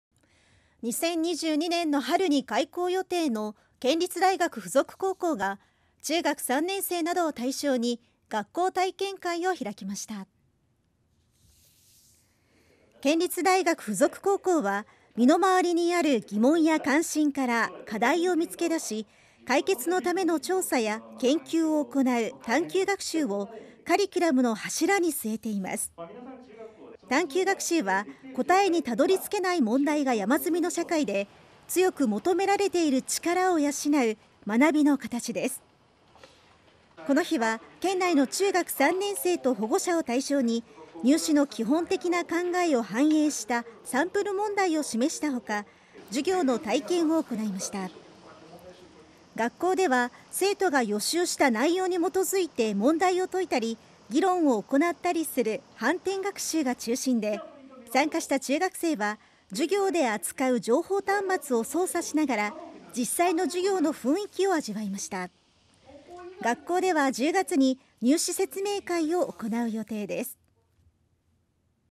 20210815-02 来春開校の県立大学附属高校が学校体験会を開催
２０２２年の春に開校予定の県立大学附属高校が、中学３年生などを対象に
学校体験会を開きました。